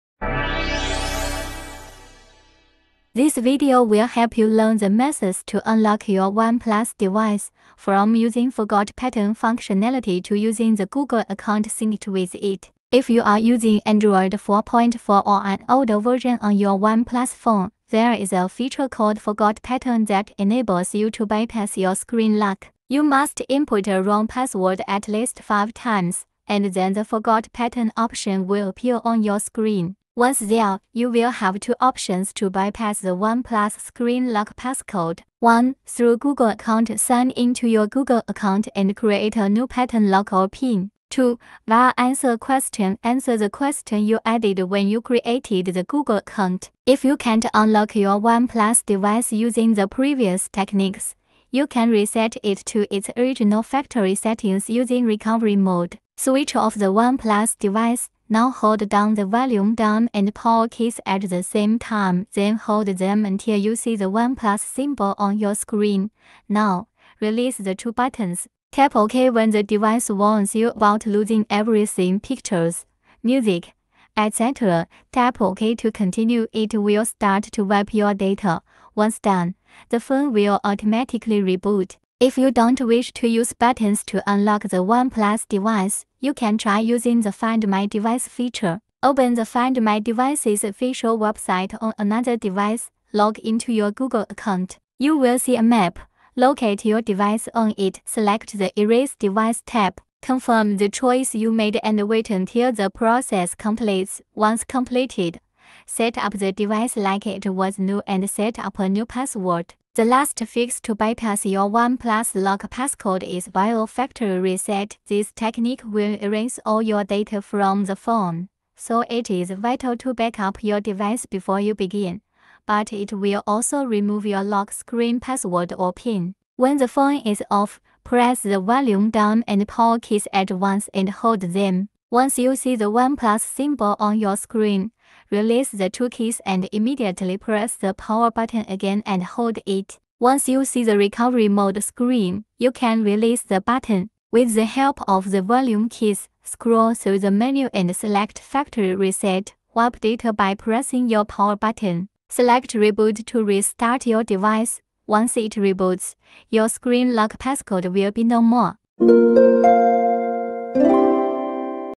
Get Unrestricted Access: How to Unlock OnePlus Phone
Is your OnePlus device locked because you can not remember the screen lock? Here are the best ways to unlock the OnePlus phone.